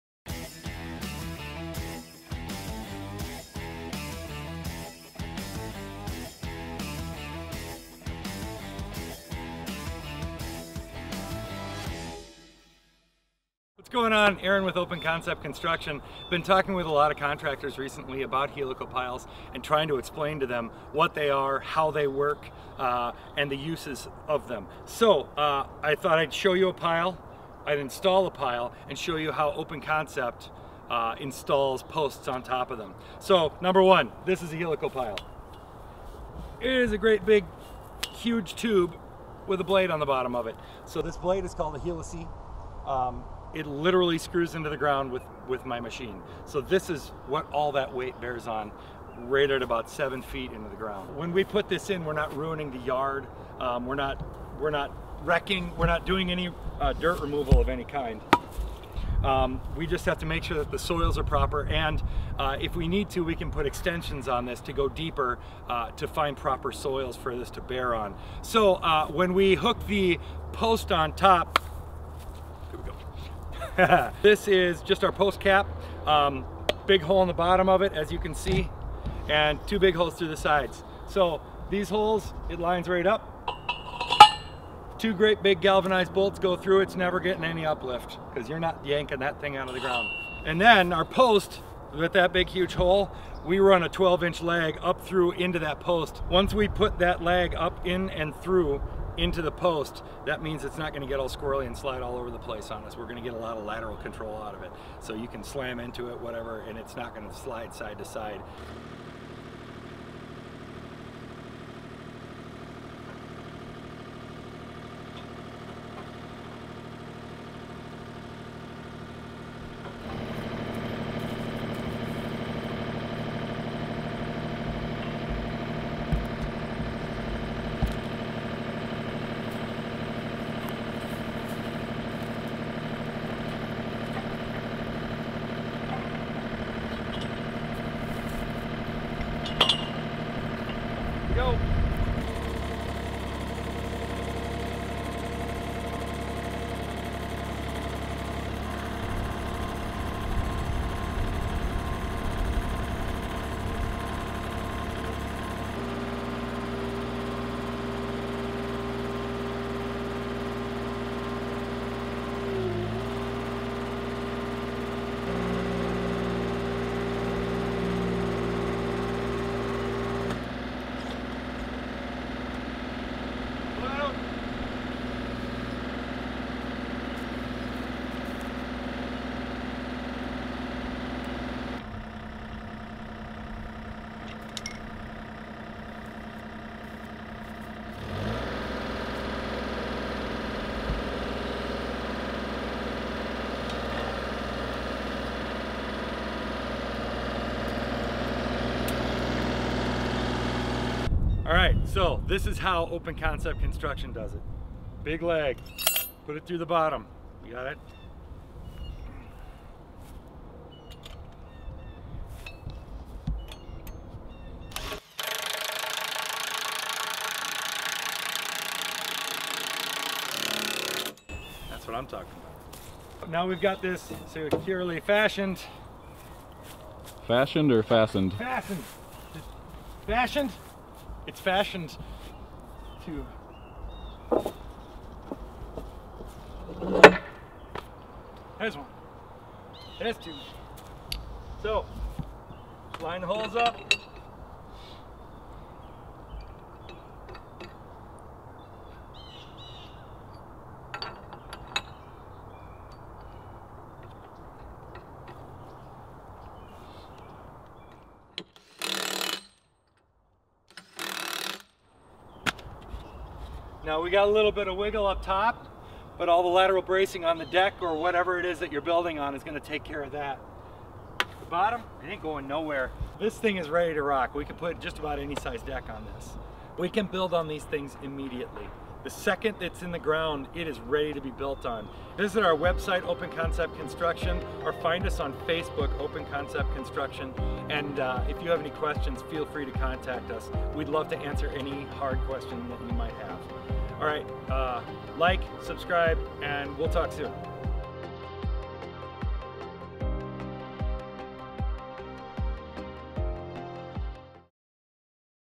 Helical Pile Basics
Aron gives us the rundown on basic construction, installation, and uses of helical screw pile footings.